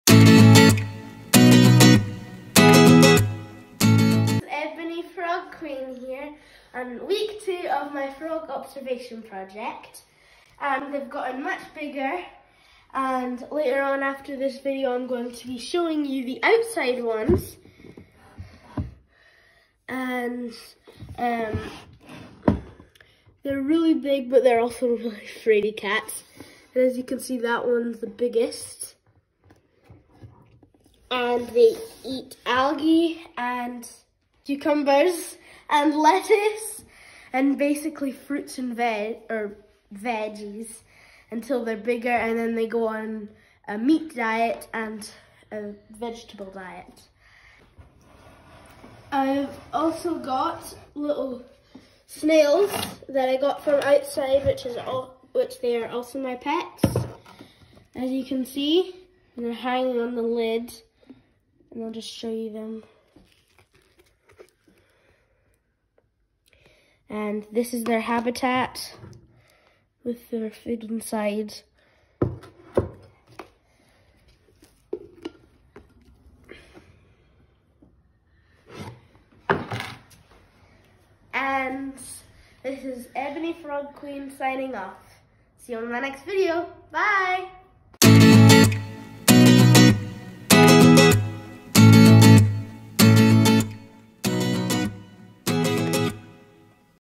MCFB Frog Vlog - episode 2
It's week two of Ebony Frog Queen's frog observation project and the tadpoles are getting bigger. There are also snails now!